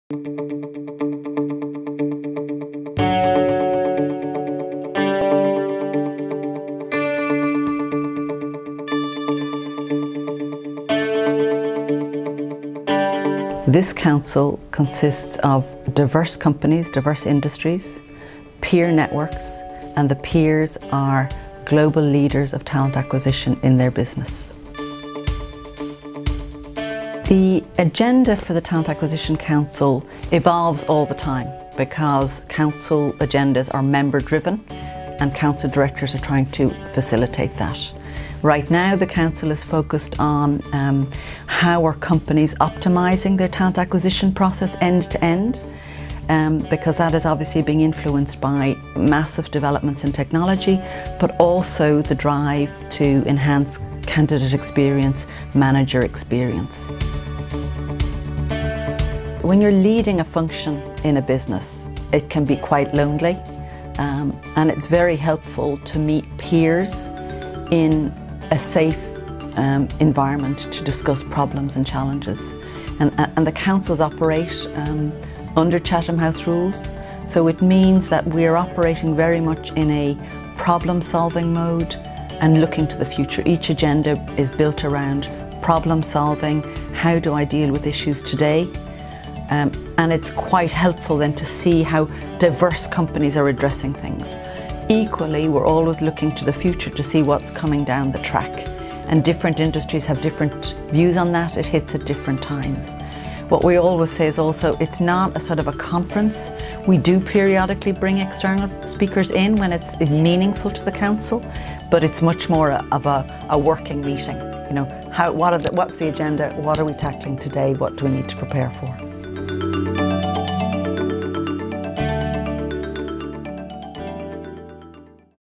Why join The Talent Acquisition Executives Council?
As talent acquisition leaders, our goal is to develop and implement strategies and tactics that enable our companies to hire the right talent at the right time. As members of this Council, we set our own agendas, conduct surveys, and participate in a variety of other activities designed to maximize the sharing of best practices and exchange of ideas. We focus both on both longer-term strategic issues and shorter-term, more immediate challenges.